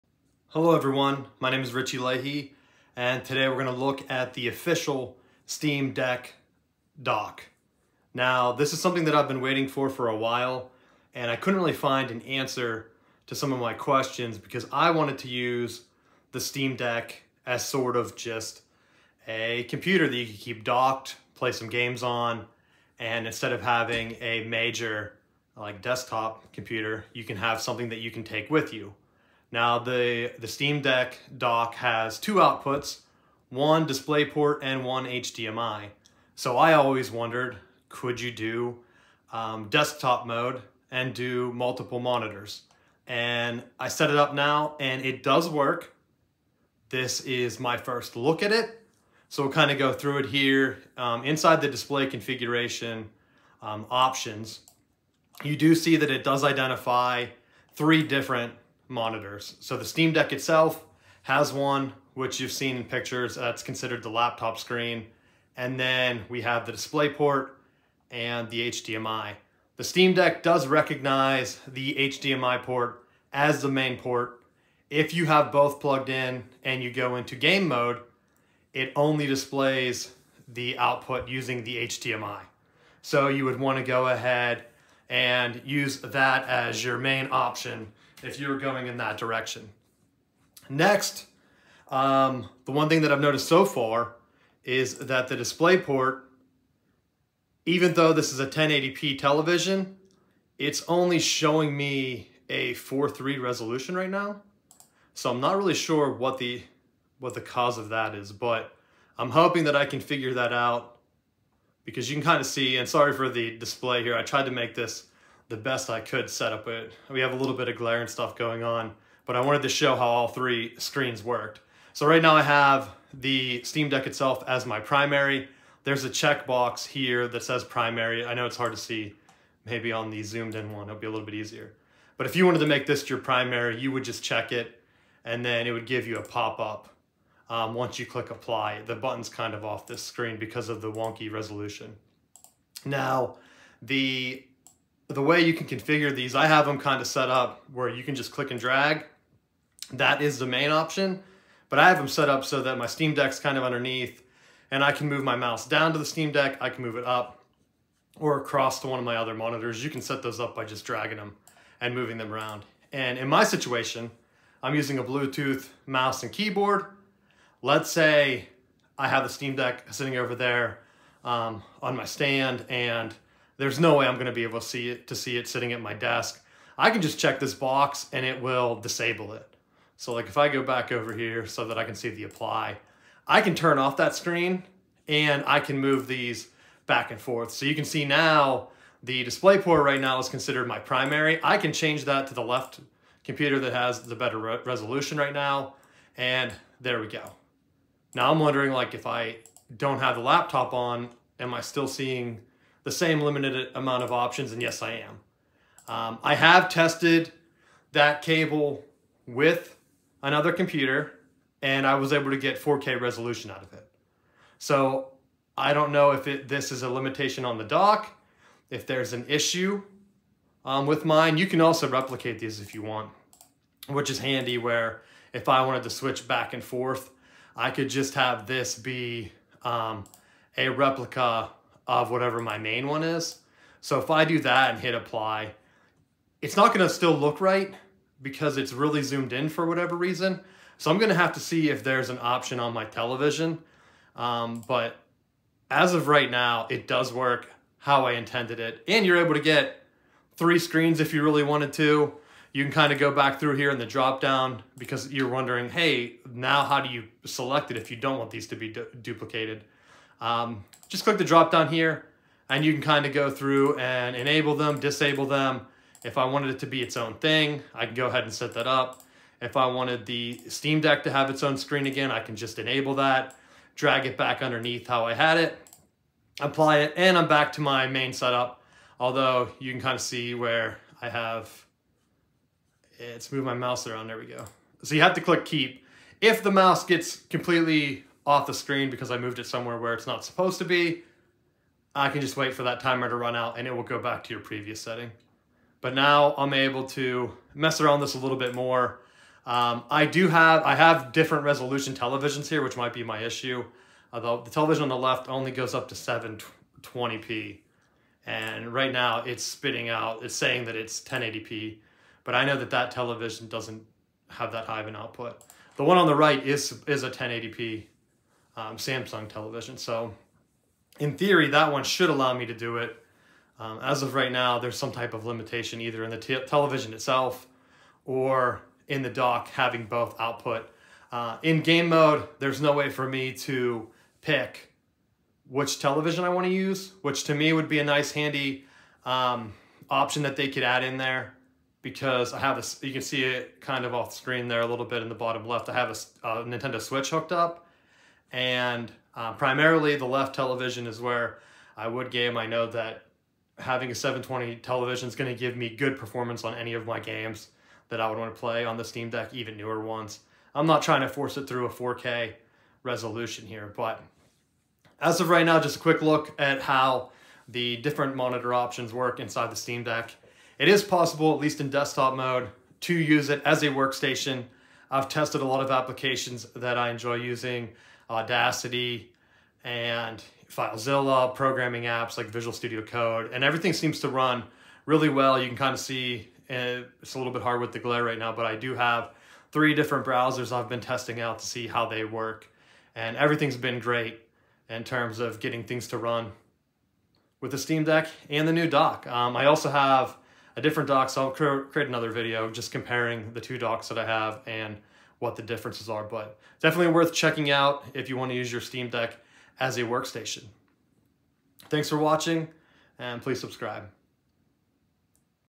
Testing the Official Steam Deck Dock with Multiple Displays
The official Steam Deck Dock can output multiple displays. In game mode, the HDMI output is currently used as the main display. Switching to desktop mode, three different displays are supported but I had issues with resolutions. The resolution options did not match the resolution of the displays and the DisplayPort was limited to a 4:3 aspect ratio. Hopefully this is something that can be fixed with a firmware update as the Steam Deck is more than capable of being used as a workstation.